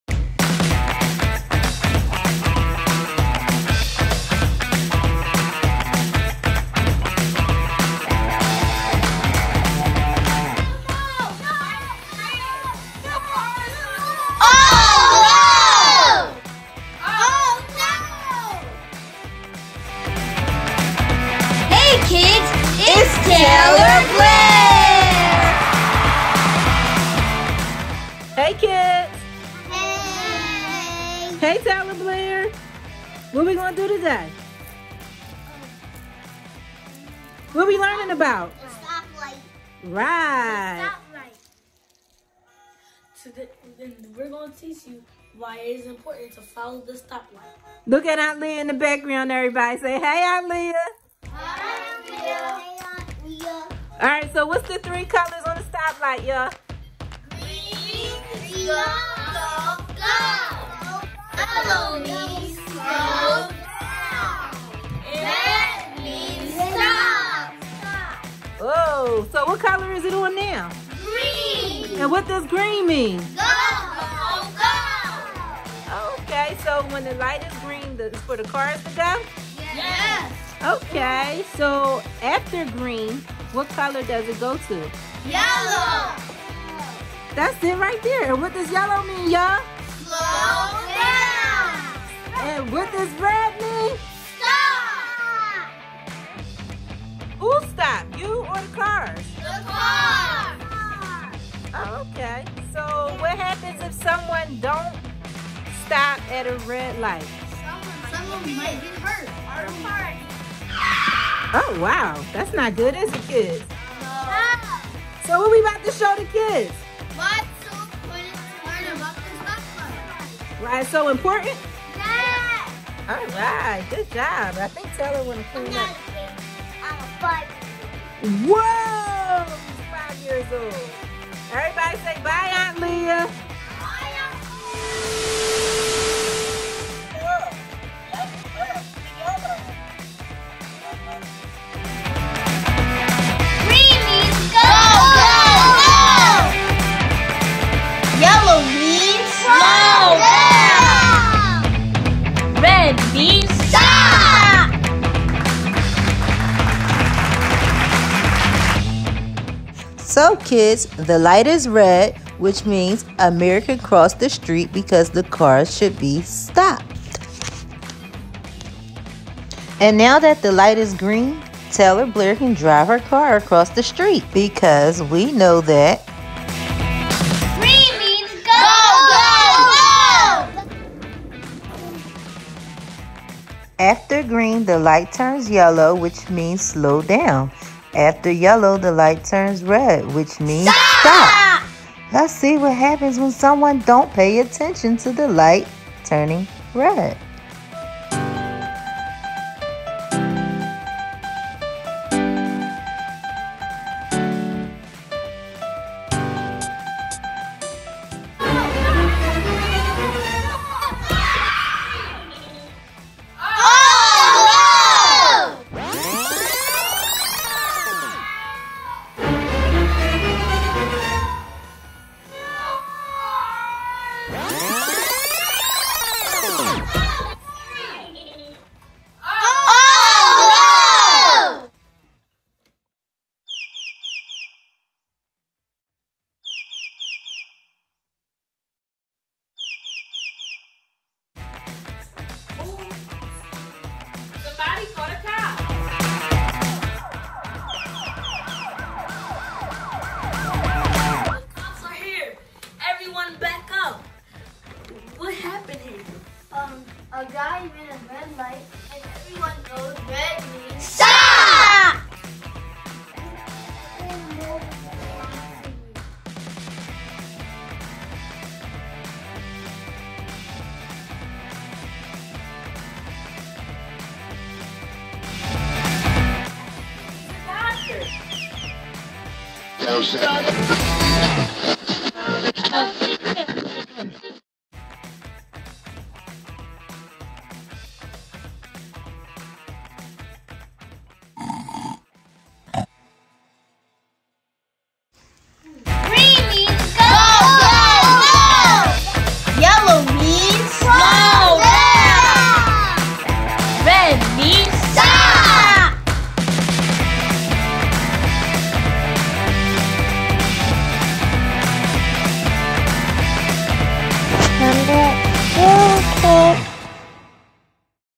Pretend Play|Learning the stop light
TaylorBlair along with her cousin’s Kaleb, Amir,Sincere, Trejour, and Ali, play out a skit which shows what can happen when drivers don’t pay attention to the traffic light.